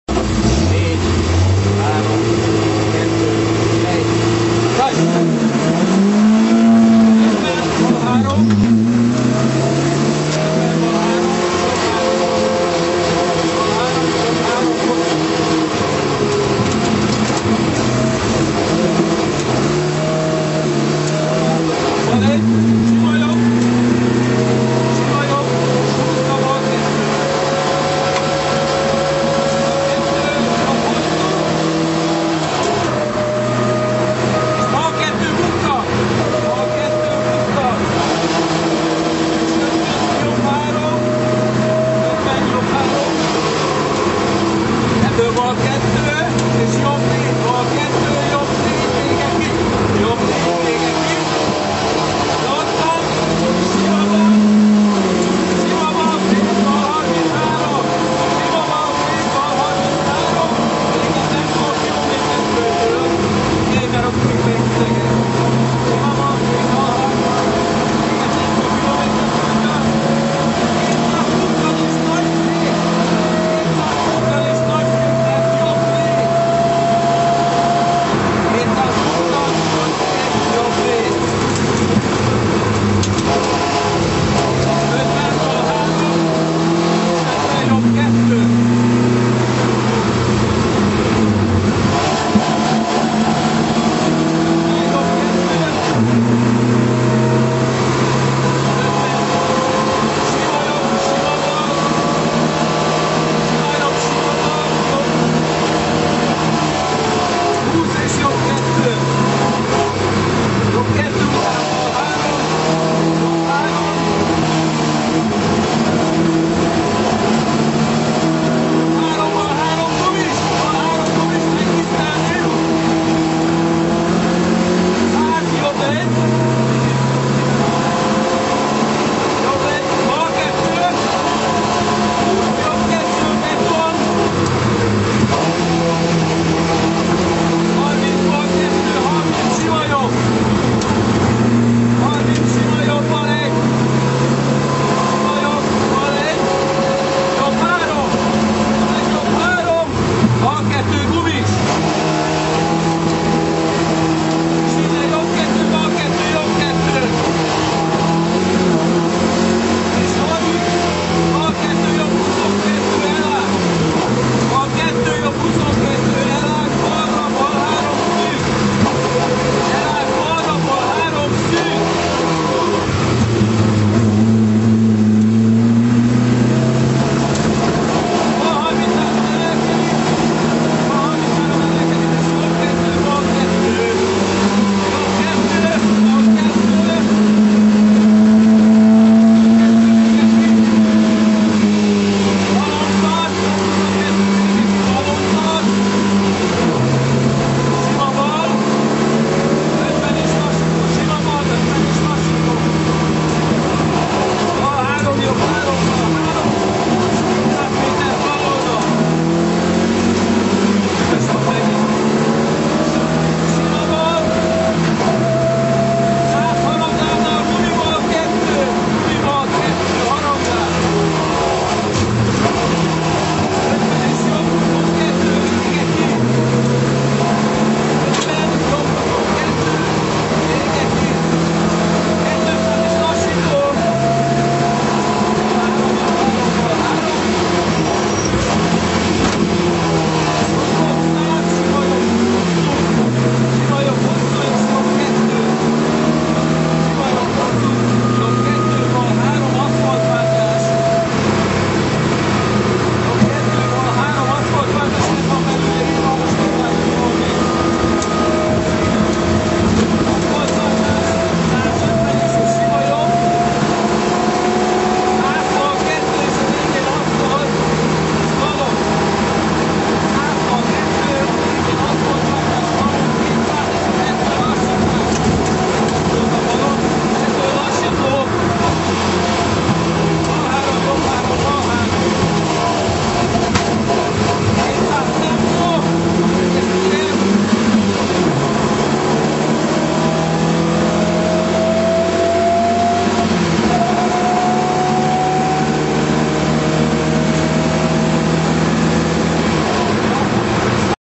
Fóti - Kiss Bakonya 2008 Gy4.
Fóti Péter és Kiss Gábor belső felvétele a 2008-as Bakonya-i rali sprint futam negyedik gyorsaságijáról.